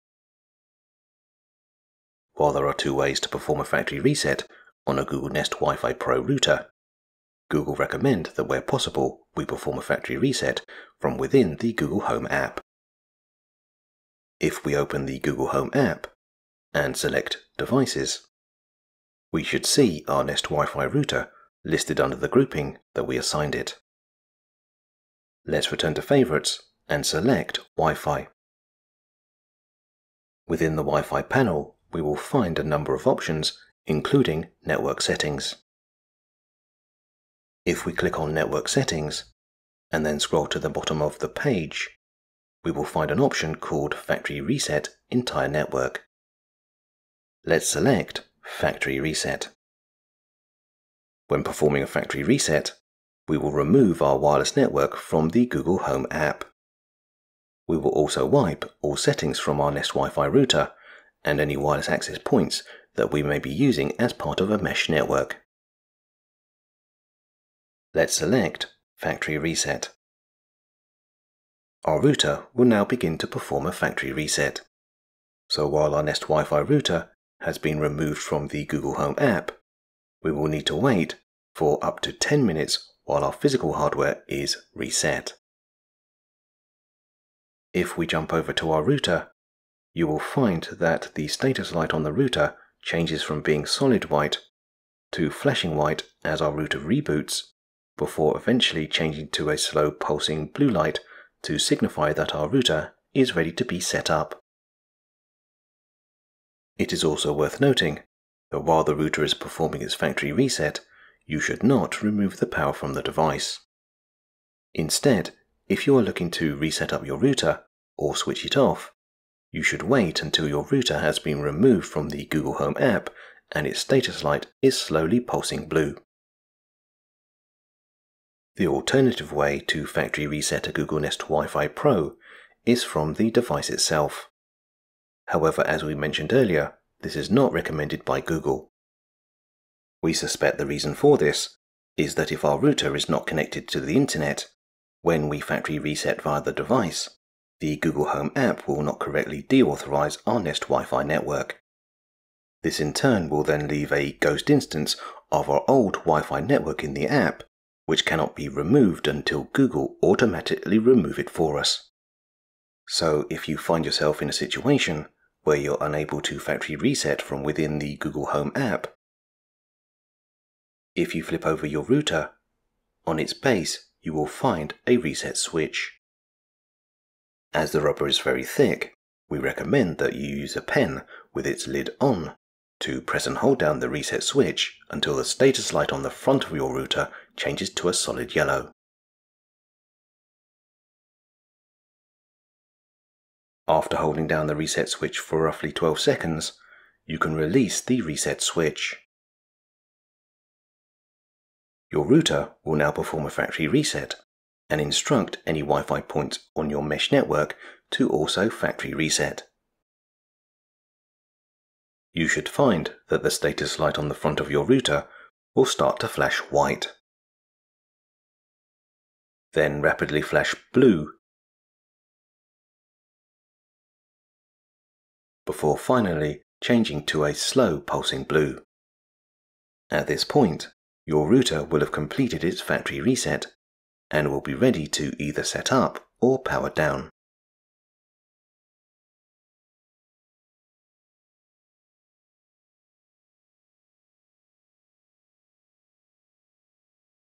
How to factory reset a Google Nest WiFi Pro
As there are two ways to factory reset a Google WiFi Pro, in this video we demonstrate both methods. However it should be noted that Google does not recommend that you perform a factory reset directly from the device.